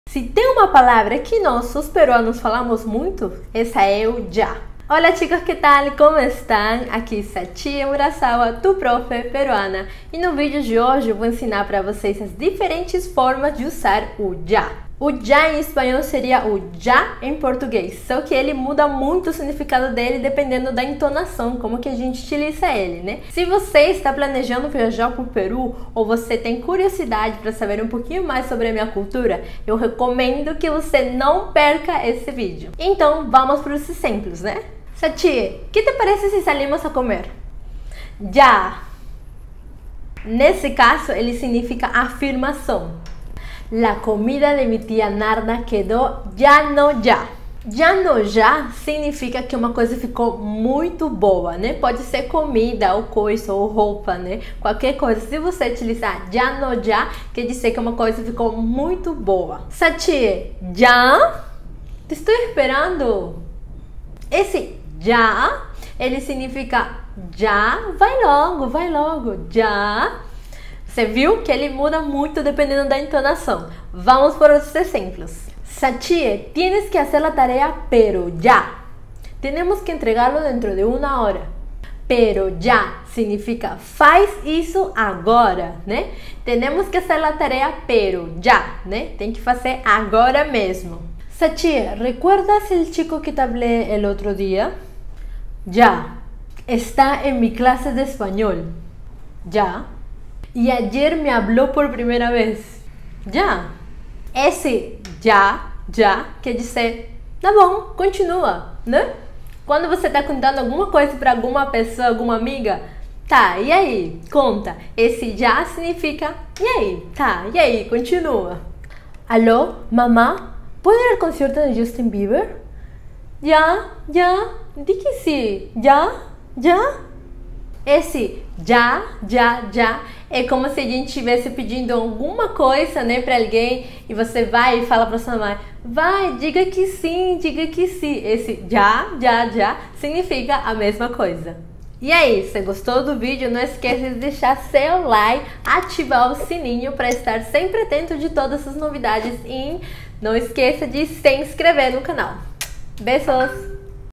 Usando "ya" no dia a dia
A palavra “ya” significa “já”, mas pode ter vários significados diferentes, dependendo do contexto. Eu separei aqui algumas frases para você incorporar no seu vocabulário:

🇪🇸 — Sachie, ¿qué te parece si salimos a comer?
— ¡YAAAA!

🇧🇷 — Sachie, o que você acha de a gente sair para comer?
— BORA!

🇪🇸 La comida de mi tía Narda quedó ya no ya.
🇧🇷 A comida da minha tia Narda ficou muito boa.

🇪🇸 Sachie, ¿ya? Te estoy esperando.
🇧🇷 Sachie, vai logo, estou te esperando.

🇪🇸 Sachie tienes que hacer la tarea, ¡pero ya! Tenemos que entregarlo dentro de una hora.
🇧🇷 Sachie, você precisa fazer a tarefa agora mesmo! Precisamos entregar em uma hora.

Pegou tudo? Marque um colega para vocês incorporarem esses usos na próxima viagem! ✈️